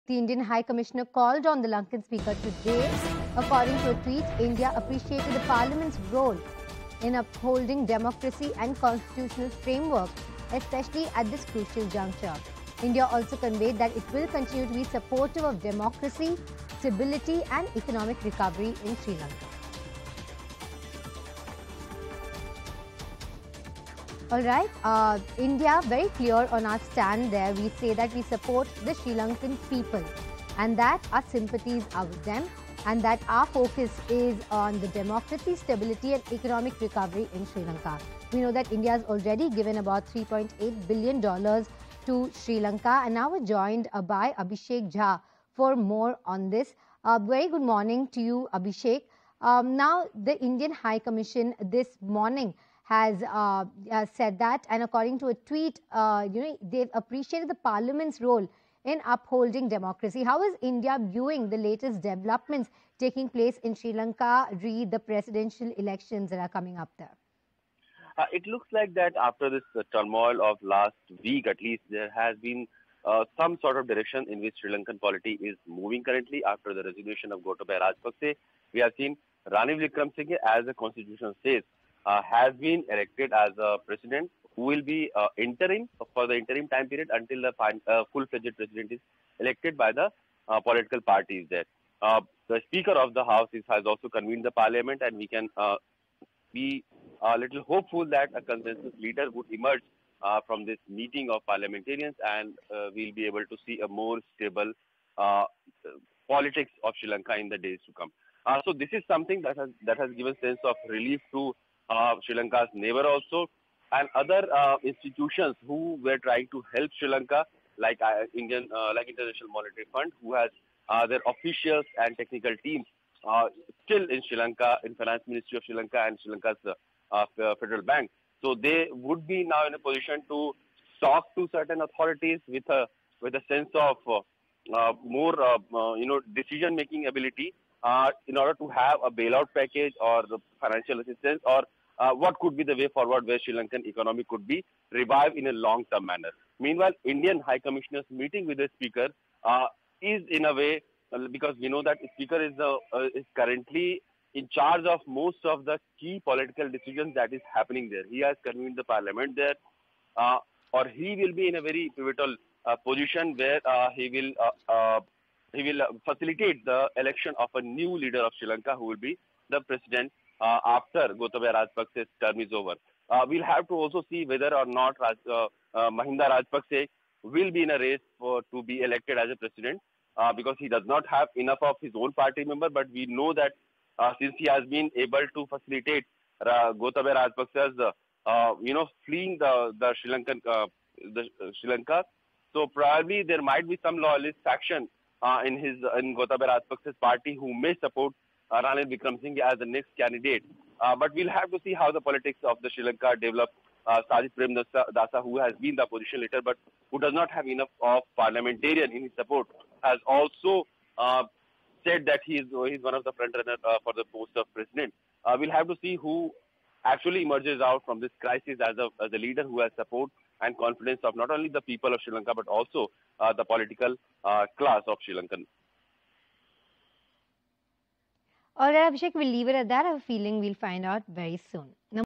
Sri Lanka Crisis | Crucial Juncture In Sri Lanka | India's Stand On Sri Lanka Crisis | English News
BREAKING SriLankaCrisis Indian envoy meet Sri Lanka speaker. He appreciate Sri Lanka Parliament’s role and support Sri Lankan democracy.